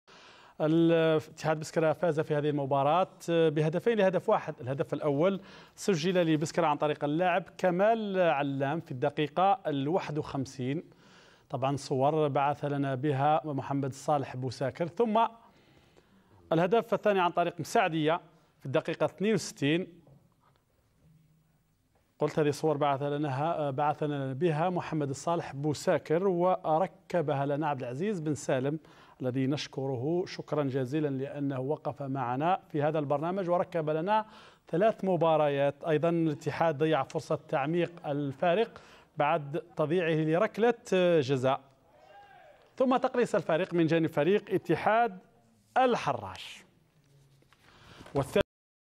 ملخص و أهداف مباراة إتحاد بسكرة 2 إتحاد الحراش 1 / USB 2-1 USMH
بطولة الرابطة المحترفة الثانية "موبيليس" لكرة القدم  الجولة 10